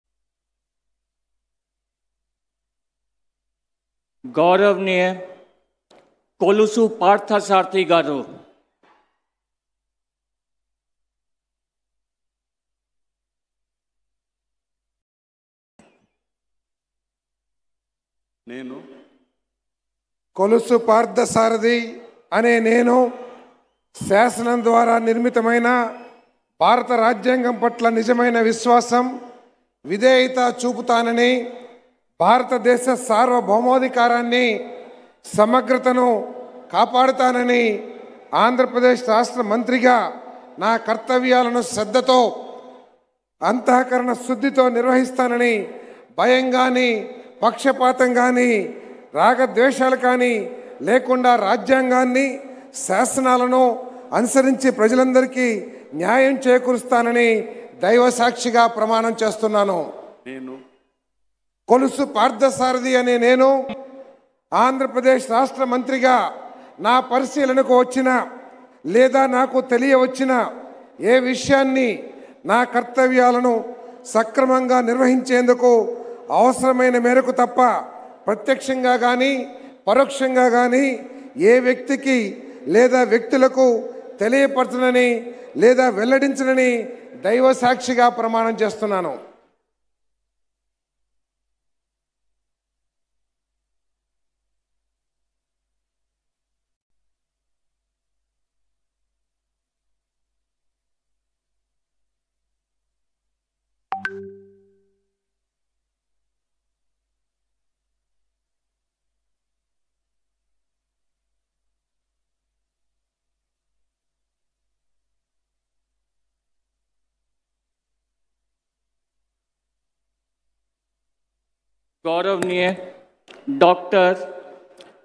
కొలుసు పార్థసారథి అనే నేను.. : Kolusu Parthasarathy Oath As Minsiter | Mahaa News
LIVE🔴: Chandrababu Live : అంగరంగ వైభవంగా చంద్రబాబు ప్రమాణ స్వీకార మహోత్సవం :  | Mahaa News